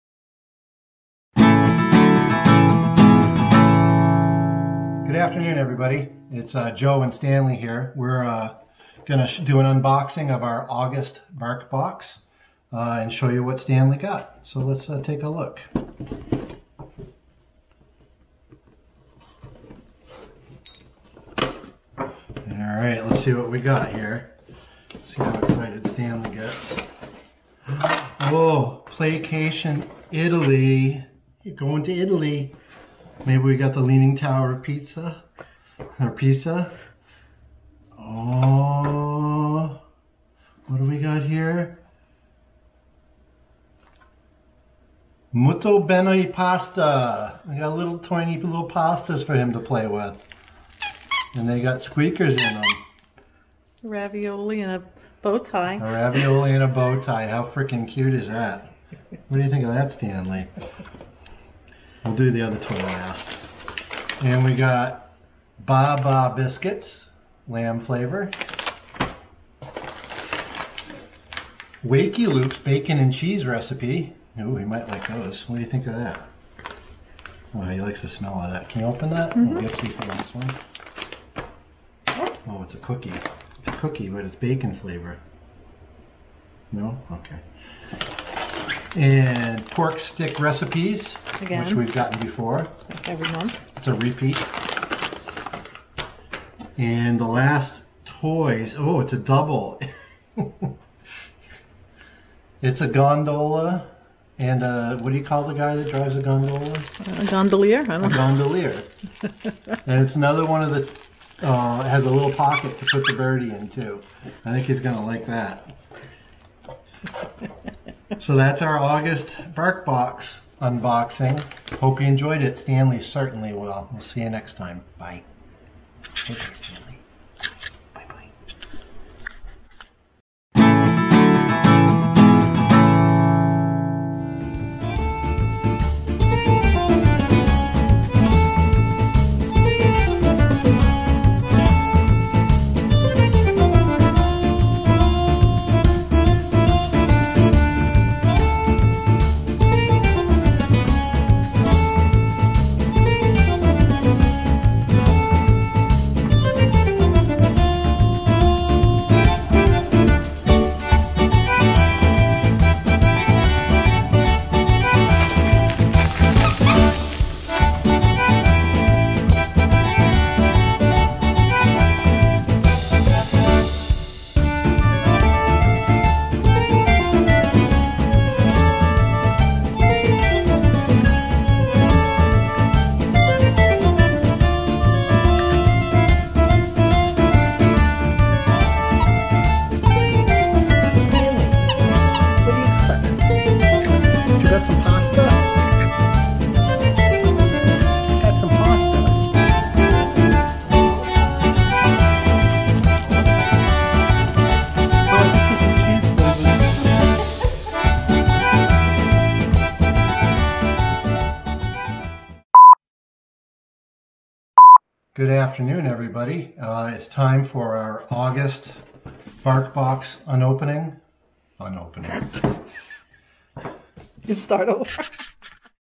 BTSO EP31 August BarkBox Unboxing!
Ciao!

This month we take a trip to Italy in Stnaley's BarkBox!  Grab your passport and a glass of vino and enjoy!